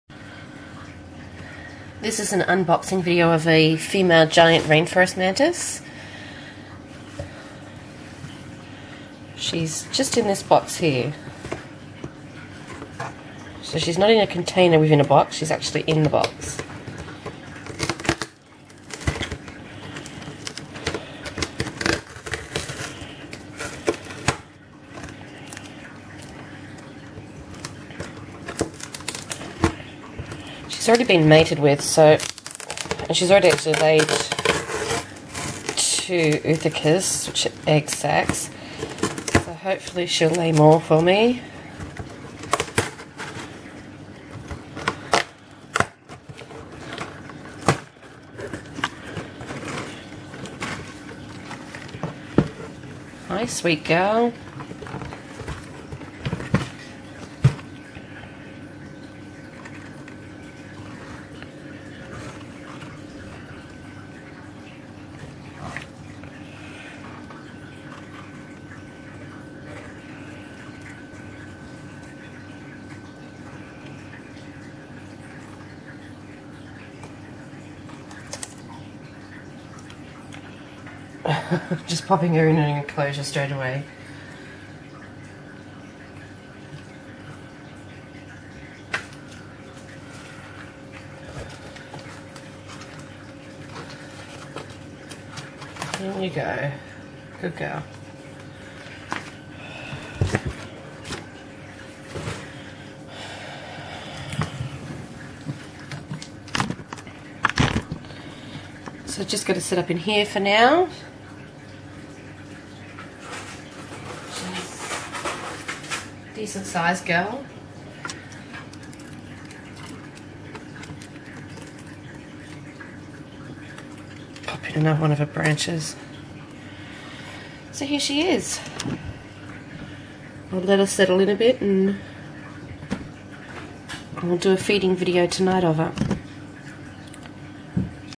Unboxing Video - Mature Female Giant Rainforest Mantis
This girl has already been mated with so and had one ootheca so hopefully she lays some more